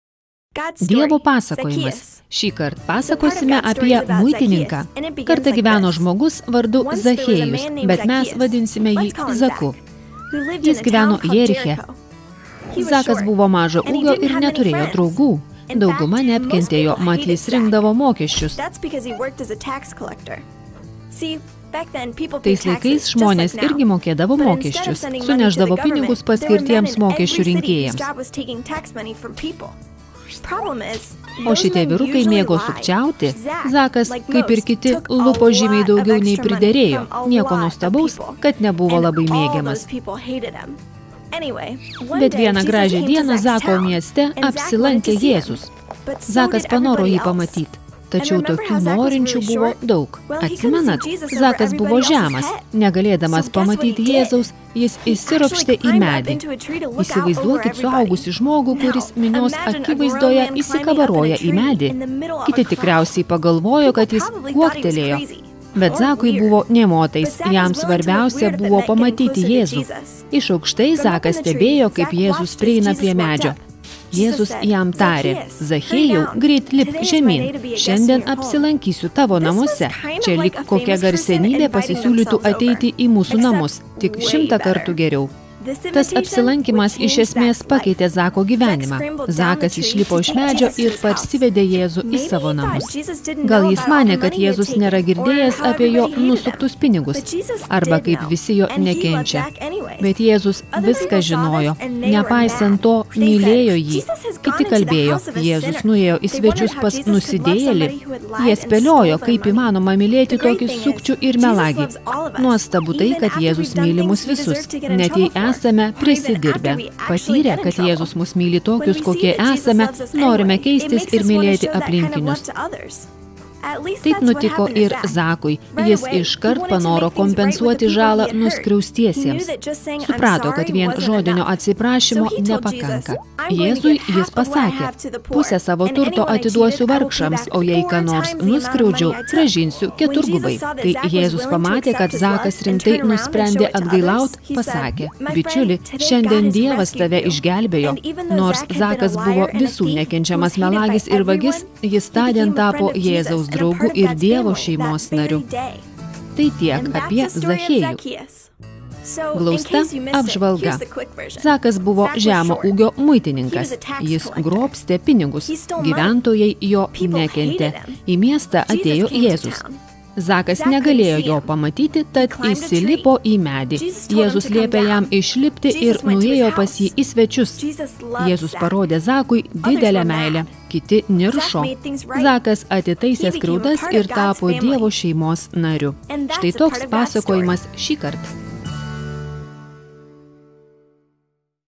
Dievo pasakojimas: Zachiejus
Zachiejus buvo mokesčių rinkėjas, kuris neturėjo draugų, nes lupikaudavo, kad taptų turtingas. Jėzus parodė jam meilę ir tai pakeitė jo gyvenimą. Tuomet jis norėjo panašiai su meile pasielgti su kitais. Visą istoriją rasite Evangelijoje pagal Luką, 19,1-9.

CrossroadsKidsClub.net